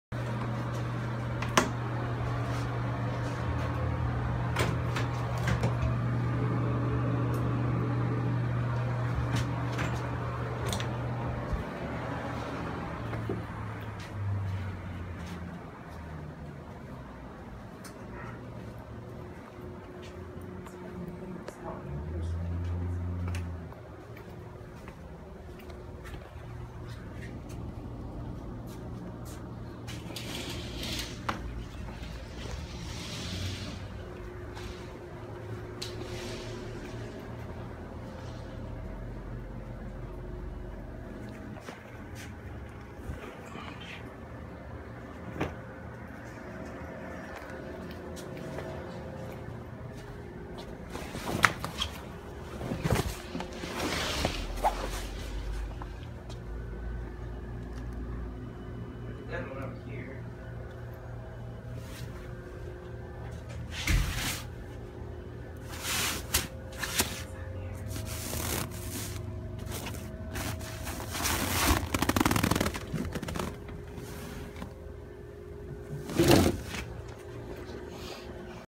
Castaway Bahama's cockroach infested
This is a small video clip of the room that we were stuck in called castaway